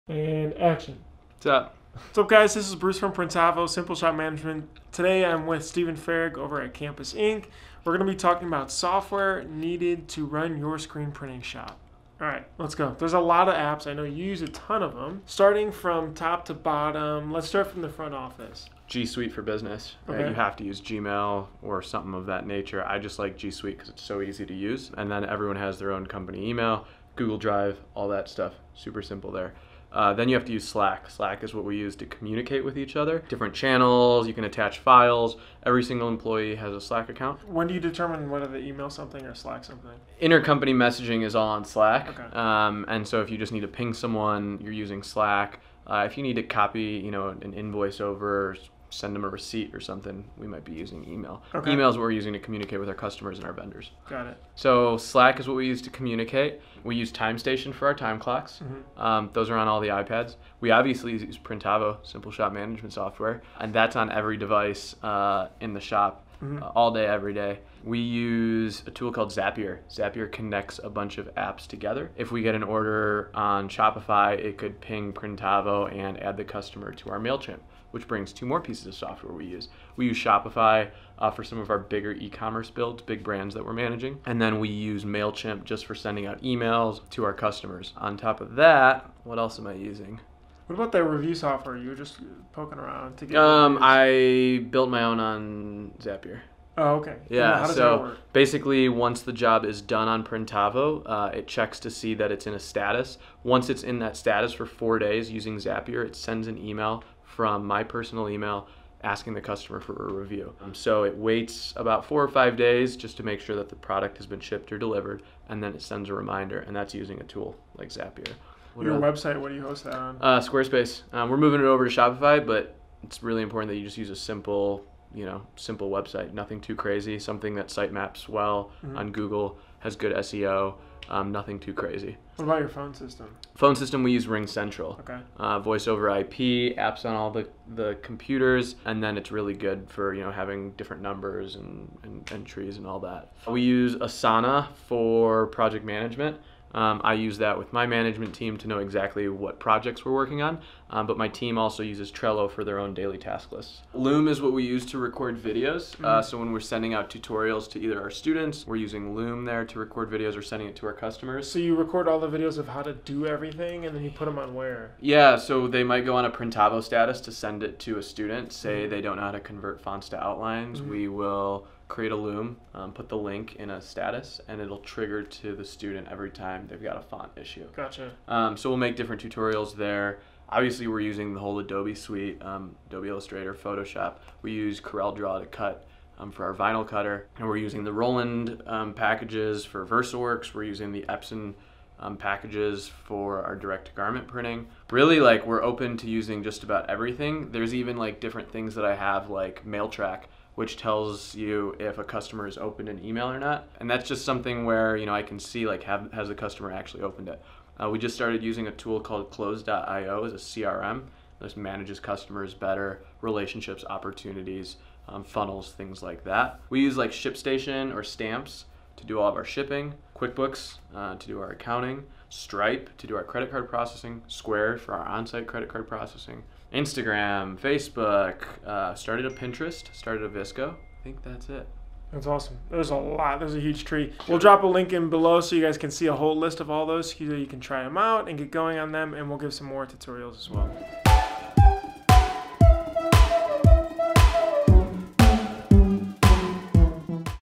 What Software Do You Need For Screen Printing?
What software do you need for screen printing? You may be surprised to find that you need a lot of different software to run your screen printing business. There's not one perfect piece of software for everything.

Read our article about the software screen printers need. It details how much it costs, what software is best for screen printing, and open-source options for budget-conscious shops

You can break down screen printing software into three areas: administrative software, production software, and sales/marketing software.

ADMINISTRATIVE SOFTWARE FOR SCREEN PRINTERS:

GSuite: Google’s suite of tools is still second-to-none in terms of availability, pricing, and quality. Whether it’s Docs, Sheets or GMail, Google has a track record for success.

Slack - This simple chat client replaces email for day-to-day communications within the team. A “Searchable Log of All Collaboration and Knowledge,” Slack dramatically improves cohesion within your team.

TimeStation - A clear time-tracking tool. Let employees know exactly how long they worked.

Asana - A project management tool, ideal for long-term projects and other business development efforts.

Trello - A powerful tasking and scheduling software with a highly visual interface. Ideal for keeping track of dozens of moving tasks at a time.

QuickBooks - A classic small business software, Quicken’s QuickBooks remains the de-facto standard accounting software for a majority of businesses within the United States. Internationally, Xero’s accounting software has gained significant traction recently. Note: Printavo syncs with QuickBooks Online for simpler accounting.

Stripe and Square - Essential for payment collection, Stripe is a low-fee online payment portal. Square is a low-fee in-person payment portal. Both are reliable and fast.

PRODUCTION SOFTWARE FOR SCREEN PRINTERS:

Printavo - Print shop management software. Handles invoicing, quoting, art and quote approvals, job scheduling, pricing, proofing and much more – specifically tailored to the custom apparel industry. Yes, that’s us!

Zapier - Zapier is a powerful tool for automation. It works by connecting different web apps together, automatically! For instance, a new inquiry on your website could ping your Slack channel and also log the inquiry into a Google Sheet. You can set custom filters and delays to get very granular control over your automations. Highly recommended!

Adobe Photoshop and Illustrator - Essential tools for crafting artwork and creating separations for screen printing, Photoshop and Illustrator pay for their subscription costs many times over on an annual basis. There are open-source alternatives (GIMP and InkScape, respectively) that can offer a no-cost entry to screen printing, but the paid tools offer better support and more refined features.

Corel DRAW - An alternative to Illustrator and Photoshop, DRAW is still a beloved image editor, with some that claim it’s superior to Adobe’s offerings. Useful for vinyl cutting!

Roland Versaworks - A print and RIP management software, typically used for helping to print transparencies for burning screens

Shipstation, EasyPost and Stamps.com - These help streamline shipping. EasyPost offers affordable shipping labels (just one penny each) while Shipstation and Stamps.com streamline the entire shipping process.

SALES AND MARKETING SOFTWARE FOR SCREEN PRINTERS:

Websites - Use a simple service like Squarespace or Shopify for discoverability on Google. Less is more! Drive customers to you, not to your website. We’ve found that a simple form is more effective than an online design tool at generating high-quality leads

RingCentral - Lets you easily track, monitor, and control complex phone trees and multiple phone numbers.

Loom - Know that the employee doesn’t know how to do the task you’re asking them to do? Make a video demonstration! A viable alternative is Screencastify.

Close.com - A highly respected CRM (customer relationship management) platform that lets you track and dissect a huge volume of data about your customers – and keep notes on them as they move through your funnel.

Steven’s attitude is simple: “We’re open to using just about everything.” If it helps the business, implement it!